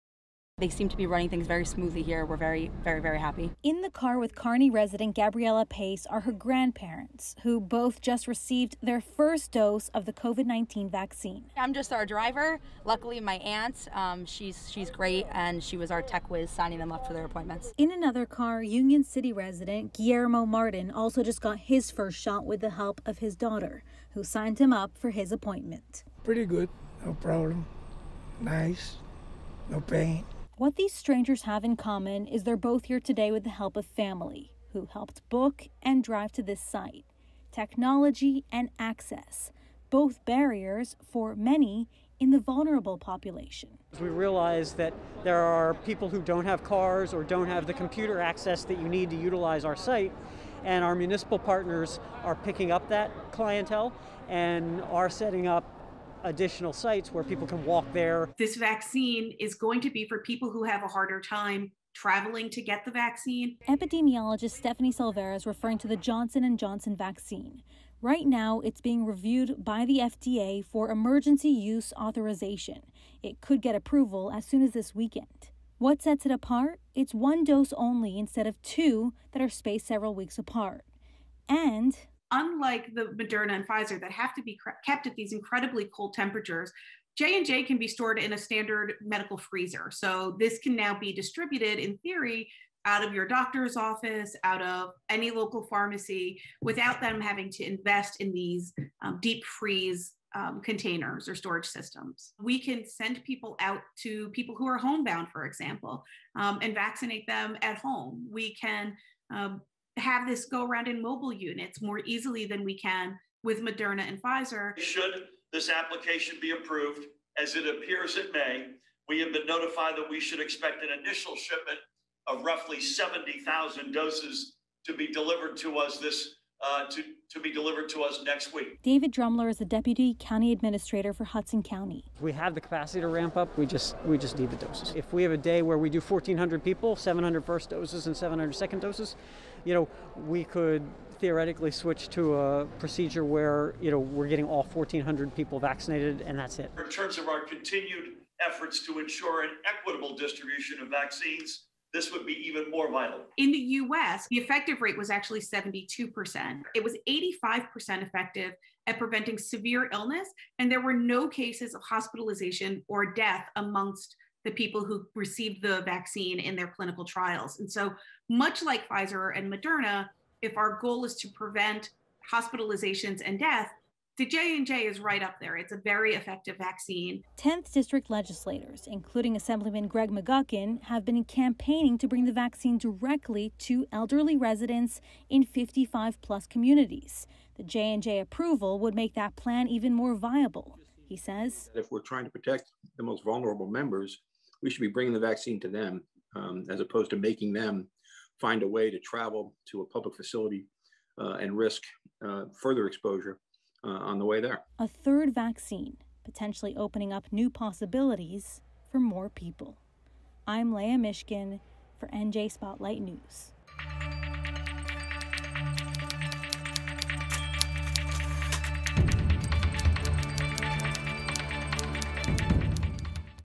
J&J vaccine could help those in hard-to-reach communities
NJ 10th district legislators, including Assemblyman Greg McGuckin, have been campaigning to bring the vaccine directly to elderly residents in 55 and older communities.